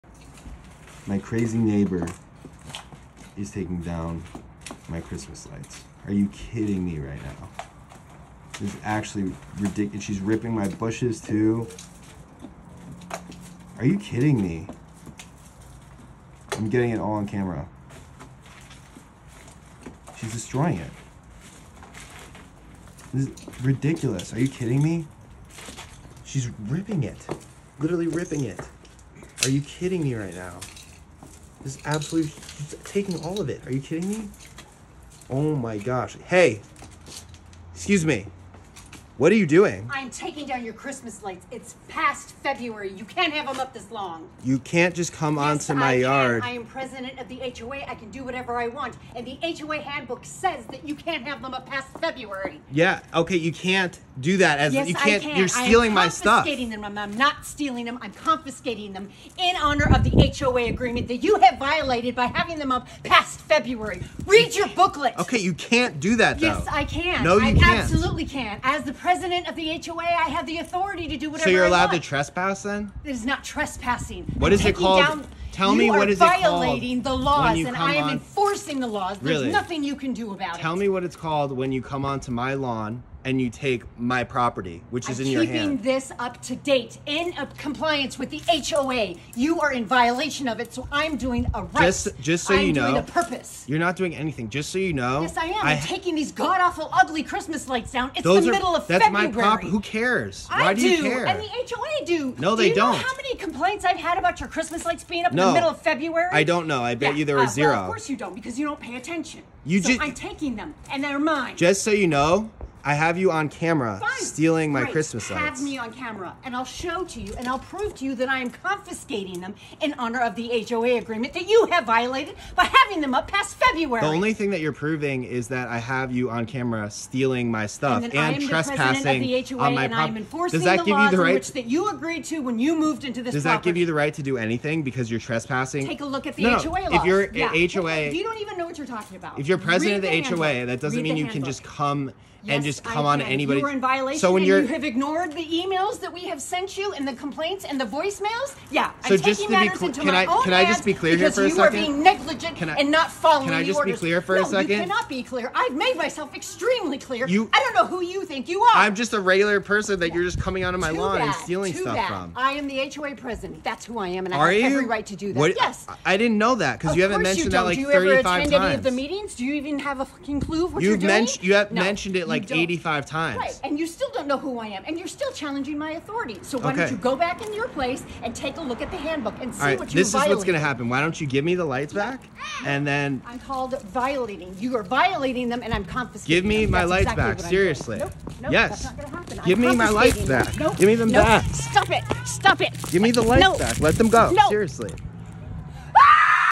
Karen Neighbor Took Down My Christmas Lights... Crazy!
Karen President of the HOA, do you think she has the authority to do that?